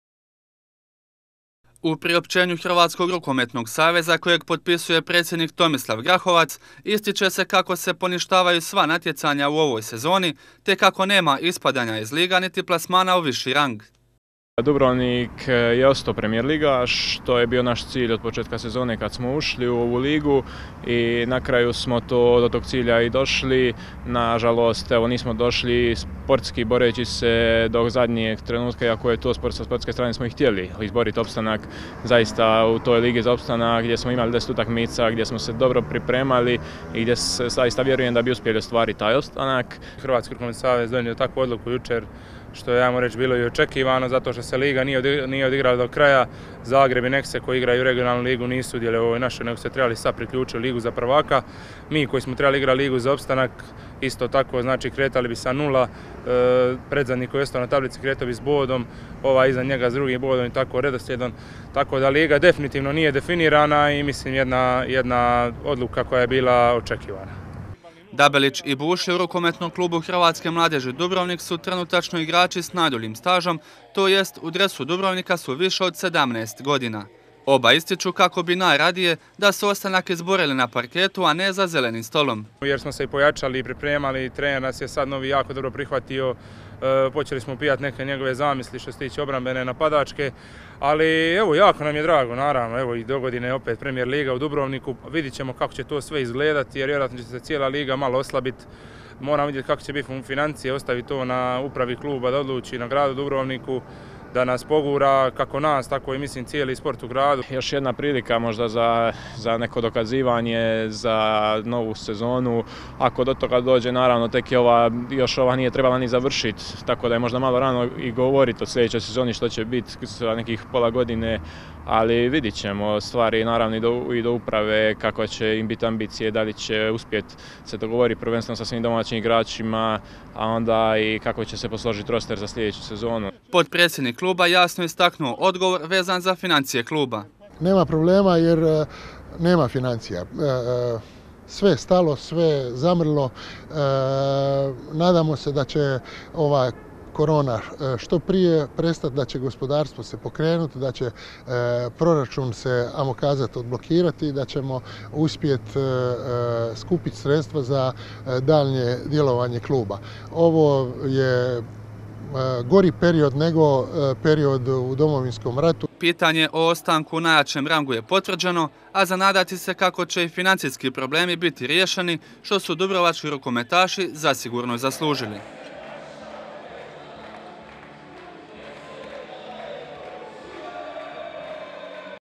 RUKOMETASI I DALJE PREMIJERLIGAŠI
Nakon što smo prije nekoliko dana objavili kako će Hrvatski rukometni savez donijeti odluku kako će se prekinuti rukometno prvenstvo – sinoć je iz Zagreba stigla i službena potvrda. U konačnici su najviše profitirali rukometaši Dubrovnika koji će i iduće sezone igrati u najjačem rangu